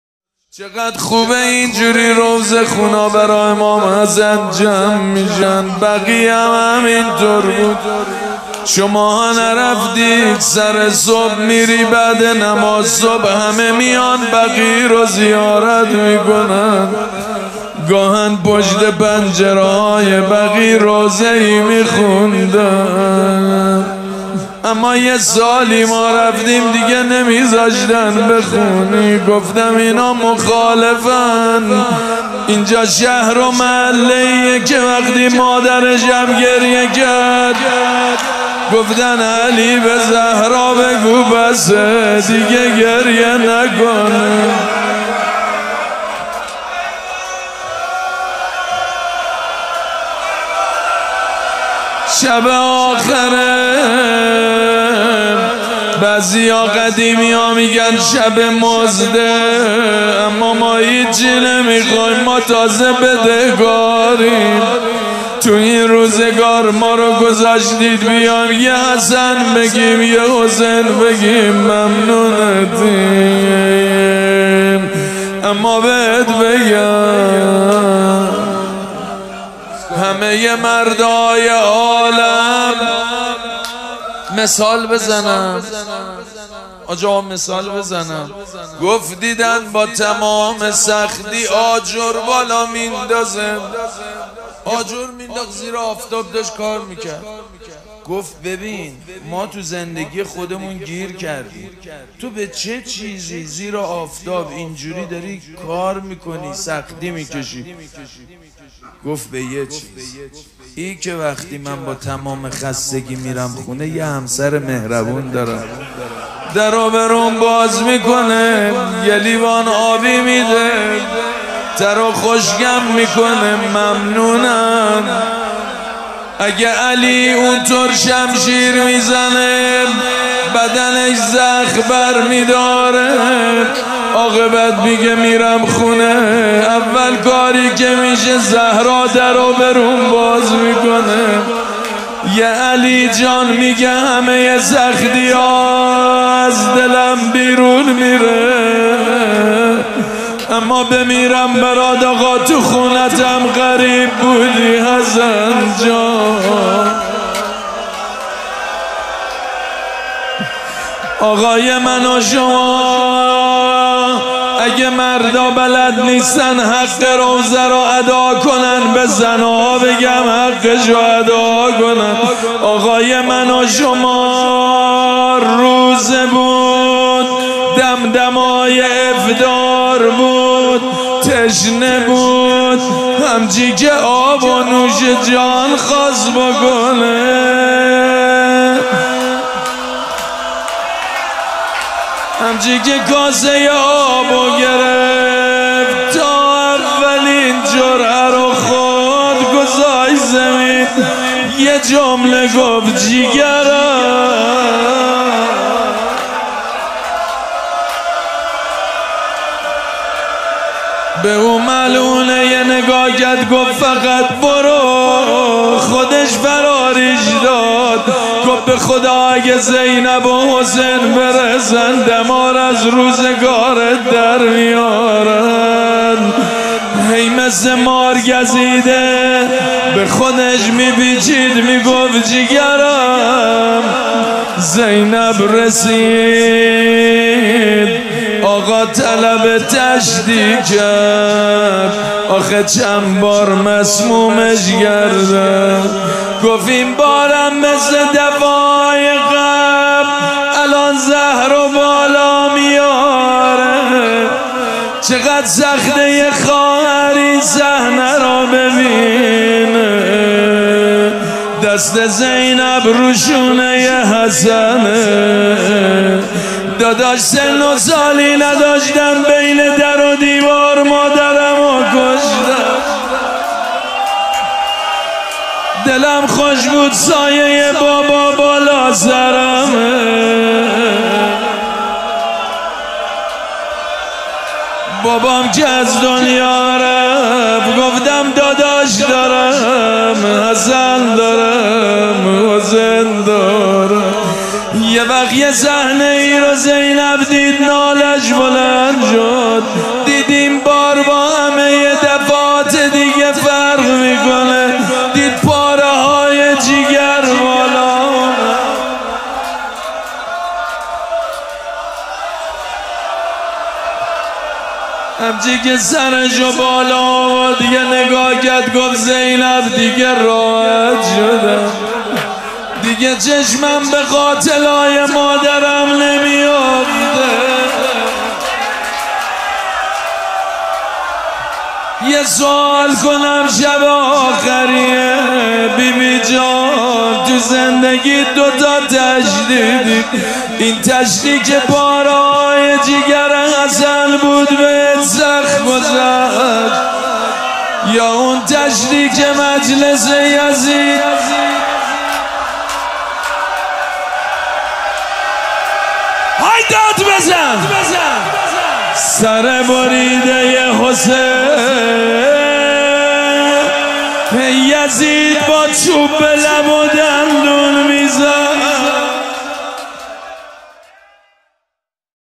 روضه امام حسن (ع) - سید مجید بنی فاطمه
#‌روضه امام حسن(ع)
️📽مراسم عزاداری شب شهادت امام حسن(ع)
🎤️حاج سید مجید بنی فاطمه
️🗓شنبه ۱۲ شهریور ۱۴۰۱ | ۶ صفر ۱۴۴۴
️📌حسینیه ریحانه الحسین سلام الله علیها